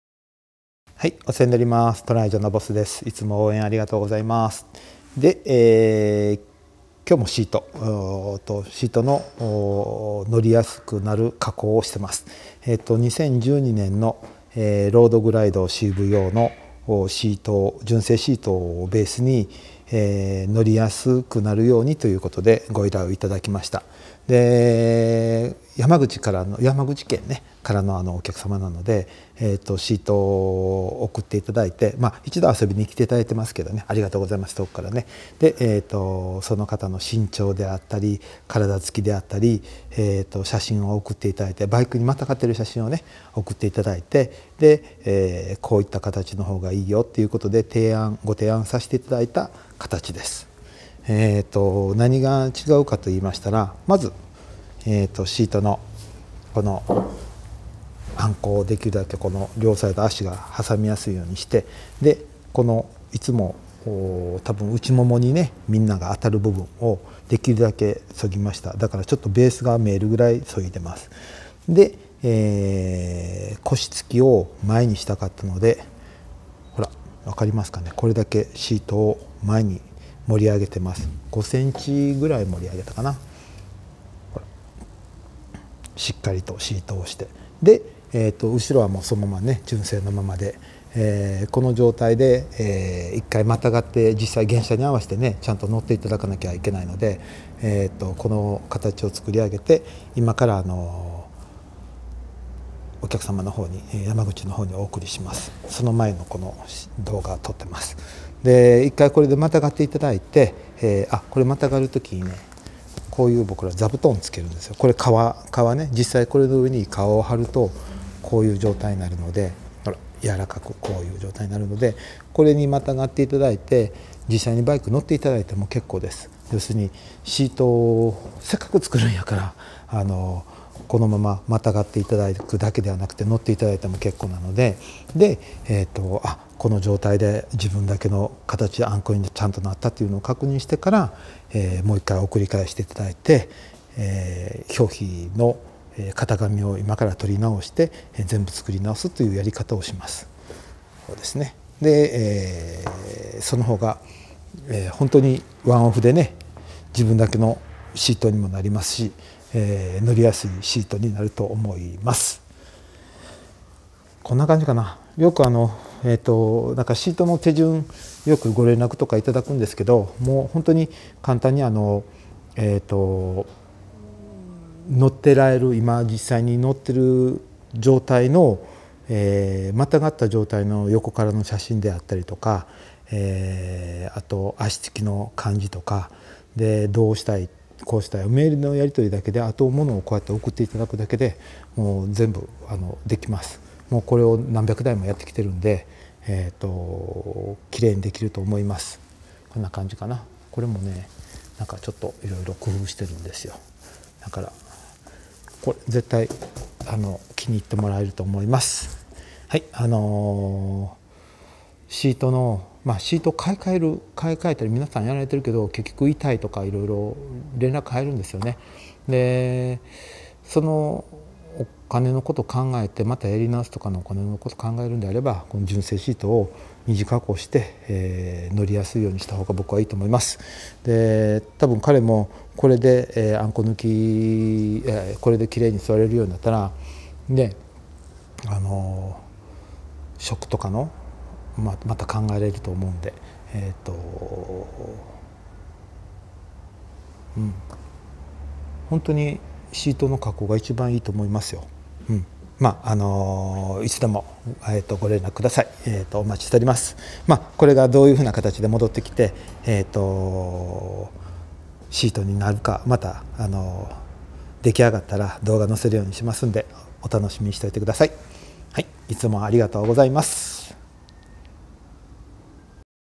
【シート】シート作りの製作工程を♪ 少しでも気持ちが伝わればうれしいです(*^▽^*)
今のシートが座りにくい・・・
腰が痛い・・・
足つきが悪い・・・
何個買い替えても辛い・・・
そんな声にお応えすべく！シートの製作工程です